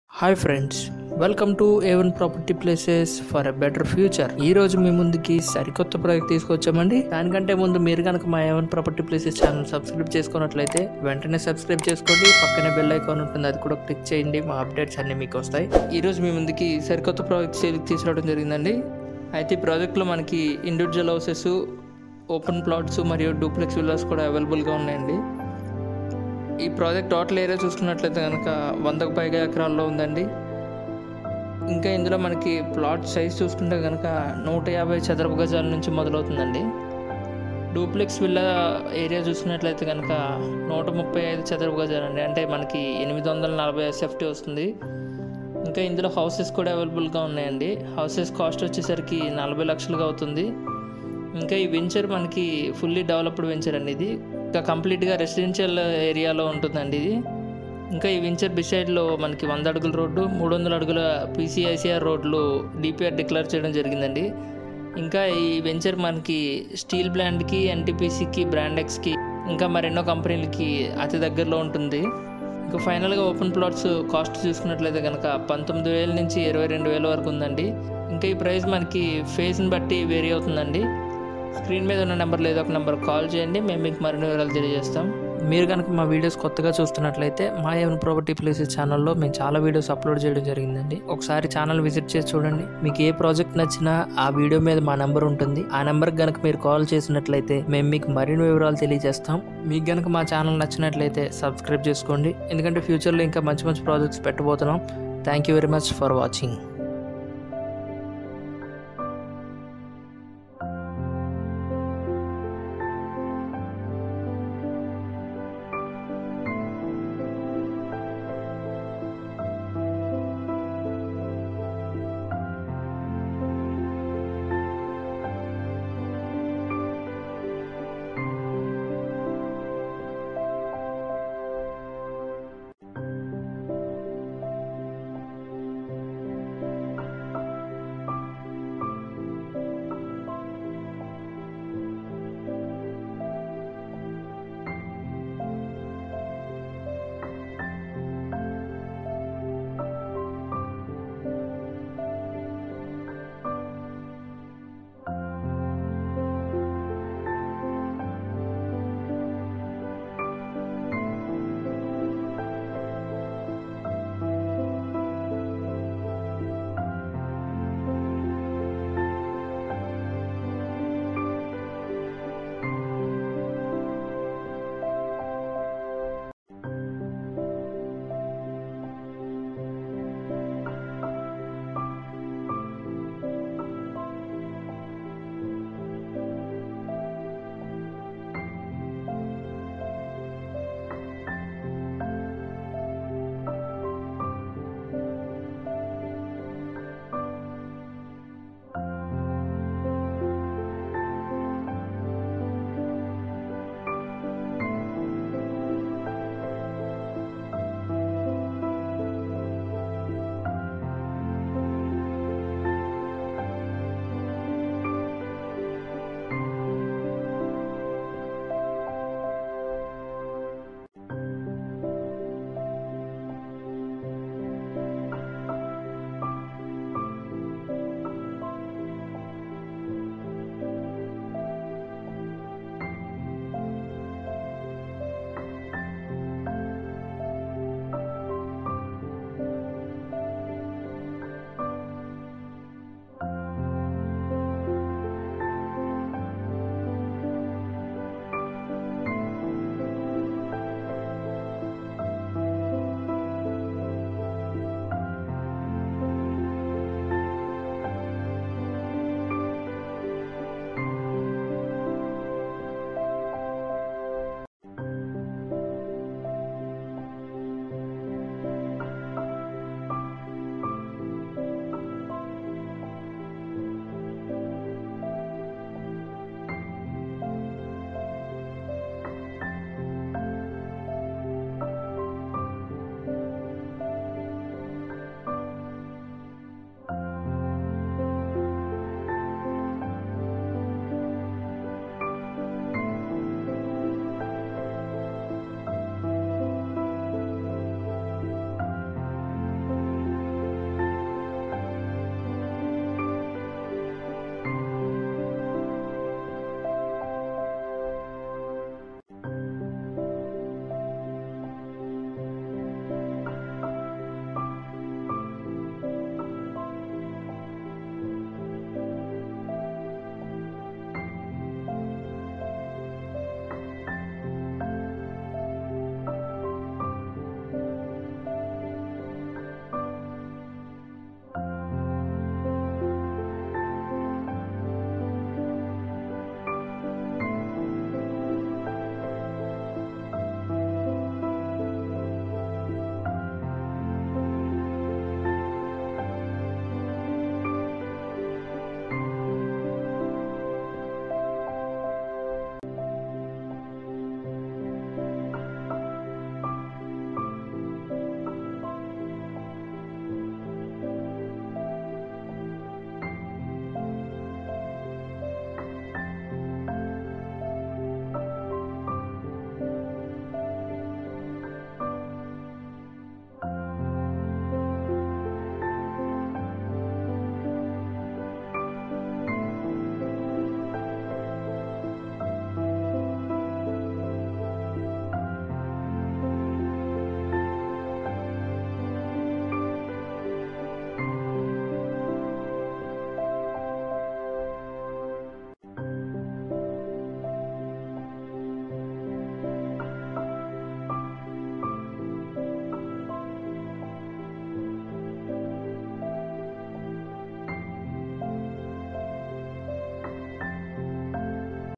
OPEN PLOTS FOR SALE | VISAKAPATNAM | A1 PROPERTY PLACES CHANNEL 2 | 9059972408 | 7382953759 | AP
హాయ్ మిత్రులారా, మా A1 PROPERTY PLACES CHANNEL2  ద్వార మీ ముందుకు మరొక వెంచర్ తిస్కోచము చుడండి అధ్బుతమైన ప్లాట్స్ కావున పూర్తిగా చుడండి నచ్హితే కాల్ చేయండి..
వివరాలోకి వెళ్తే ఈ ప్రాజెక్ట్.
FULLY DEVELOPED VENTURE.
ఆంధ్రప్రదేశ్ లోని విశాఖపట్నం దగ్గరలో అచ్యుతాపురం లో ఉన్న ప్రాజెక్ట్ - 100+ ACRES, VMRDA లేఅవుట్ లో ఓపెన్ ప్లాట్స్, INDIVIDUAL HOUSES, DUPLEX HOUSES  దగ్గరలో NTPC, STEEL PLANT, MANY MORE COMPANIES, దగ్గర అందుబాటు లో ఉన్నాయ్, ఓపెన్ ప్లాట్స్ - 150 SQ YDS ON WARDS, DUPLEX HOUSE - 135 SQ YDS - 840SFT, SIMPLEX HOUSE - 40 LAKHS, 100FT ROAD, 300FT ROAD PCPIR ROADS ALSO DECLARED IN DPR అందుబాటు ధరలో కేవలం 19000/- PER SQ YD TO 22000/-
LIMITED PLOTS AVAILABLE
మరిన్ని వివరాల కోసం - సైట్ విజిట్ కొరకు కాల్ చేయండి  - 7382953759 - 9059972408.